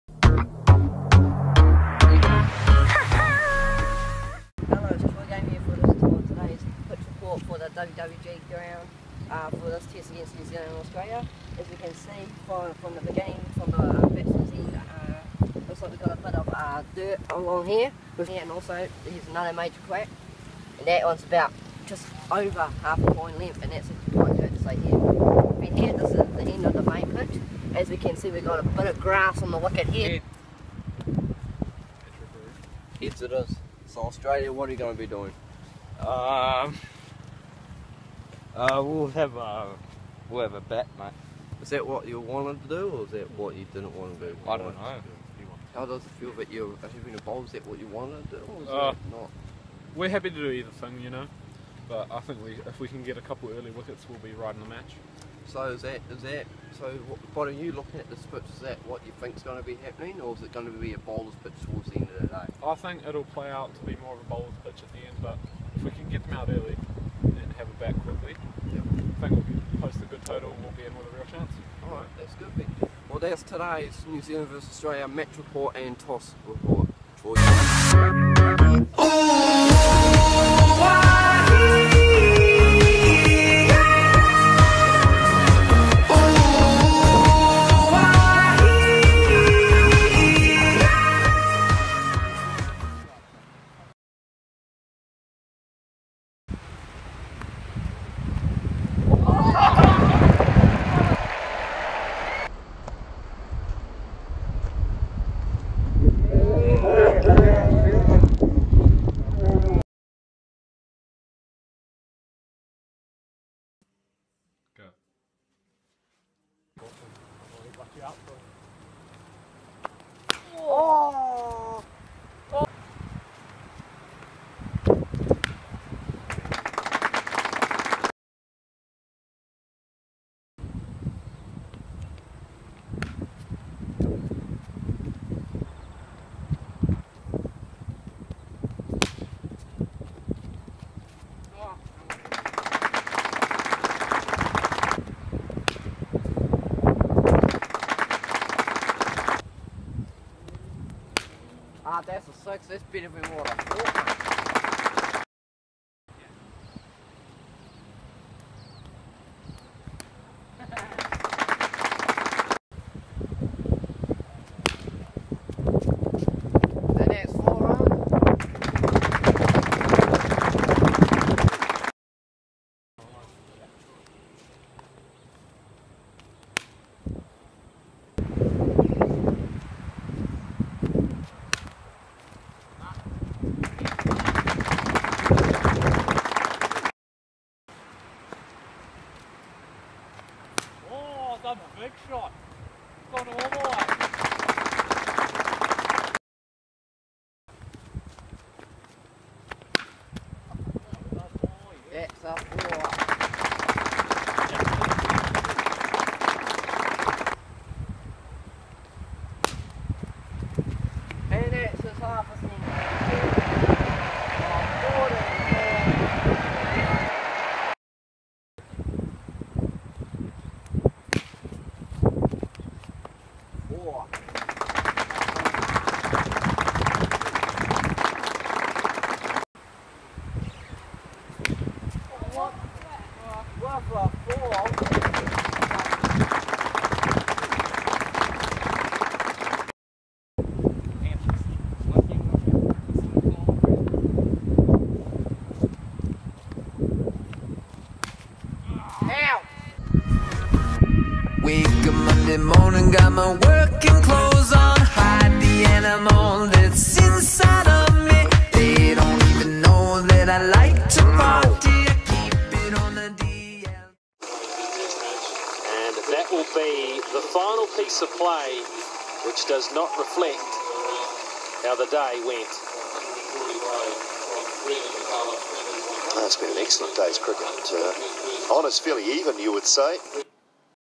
NZ vs AUS First Backyard Test Morning Session
Coverage from the first session of the first backyard cricket test between Australia and New Zealand including some spectacular drives to enjoy